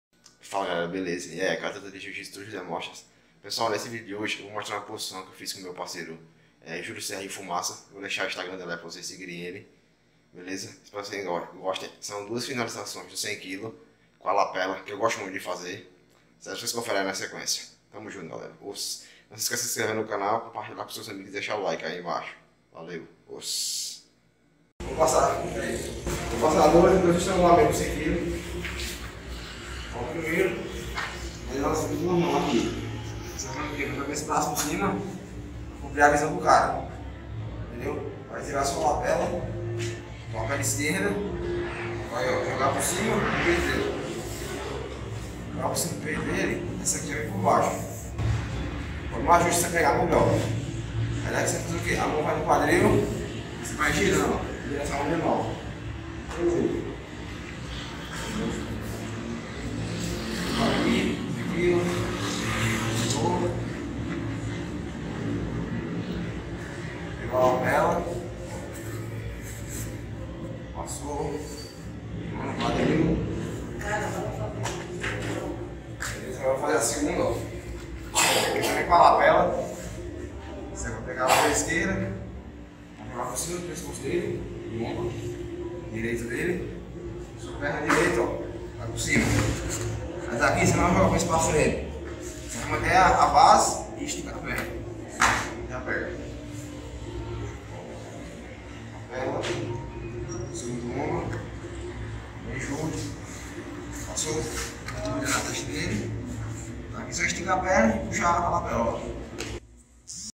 2 Finalizações dos 100kg com a lapela no jiu-jitsu - Tullius Demosthenis
Fala galera, no vídeo de hoje vou mostrar 2 finalizações dos 100kg que uso muito em meus treinos e competições espero que gostem, se inscrevam no canal, deixem seu like e compartilhem, Oss!!!